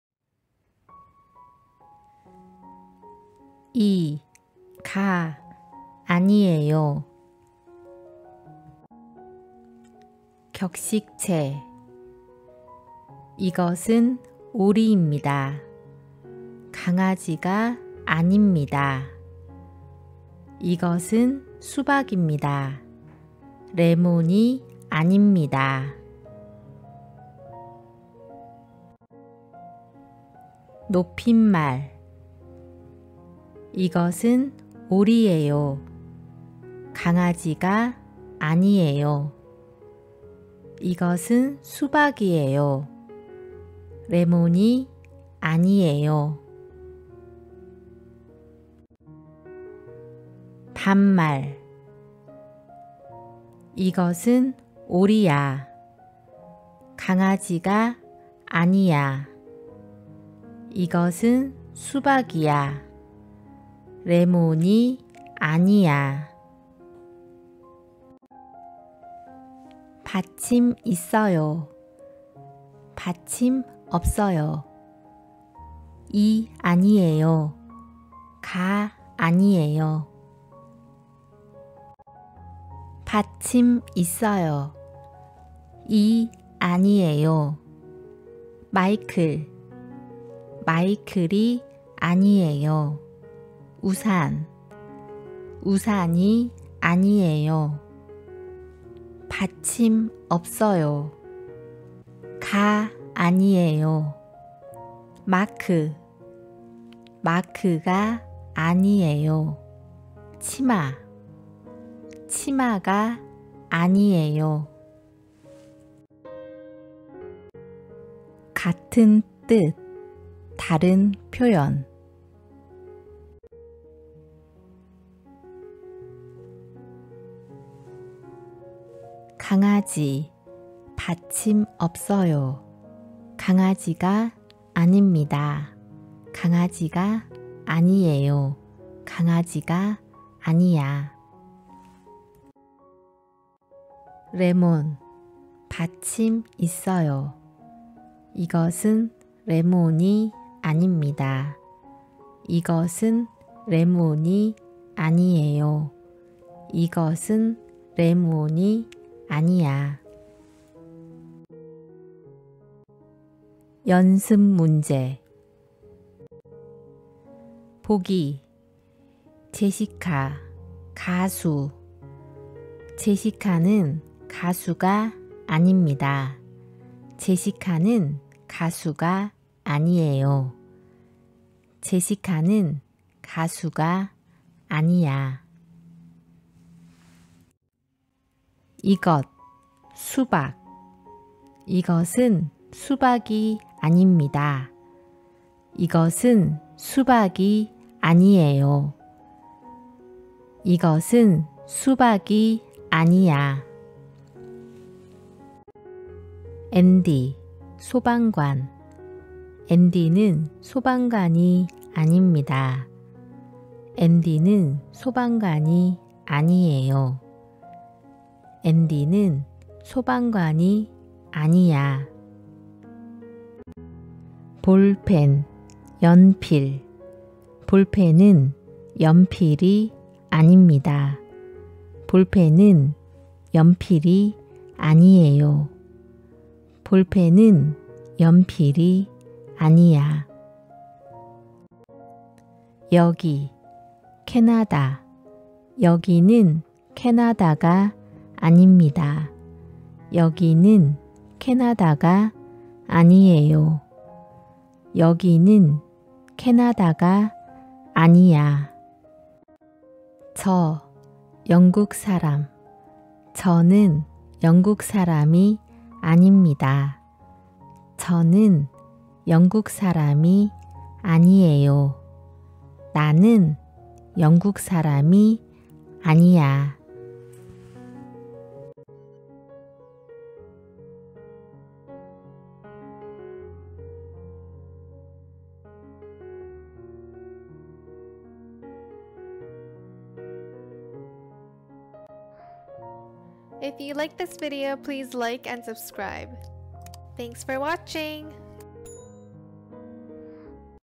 30) 한국어 문법 4 - 이/가 아니에요, Korean Grammar for Beginner, 초급 한국어 문법, 자막(ENG SUB)
Learn Korean Grammar 이/가 아니에요
Korean Grammar for Beginners Class
외국인을 위한 초급 한국어 문법
It's not _____.
____이/가 아닙니다. 아니에요. 아니야.
Formal style & Informal style
Polite form & Casual form

Learn all Korean letters in 30 minute